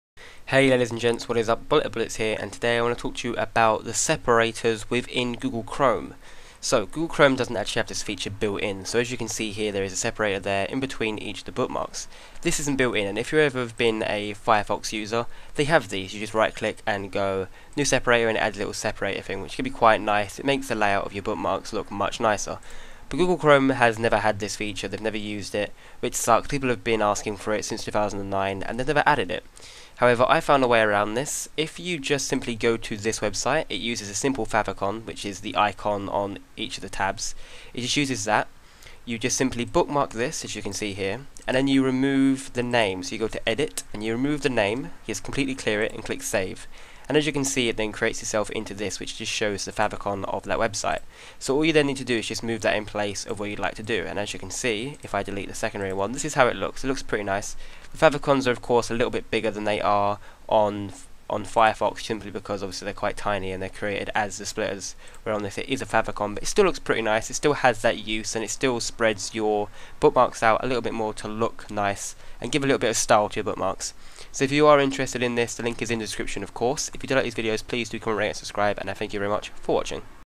How to Add Bookmark Separators in Google Chrome
Then right click the bookmark, select 'edit', remove the title/name of it, then click 'save'.  And you're done! Just copy & paste that as many times as you want a separator.